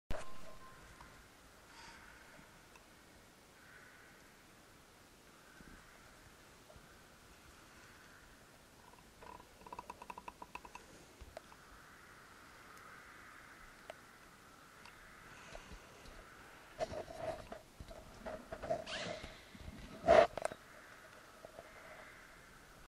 Kościół w Głogówku
Wnętrze tego obiektu sakralnego, widać przez chwilkę organy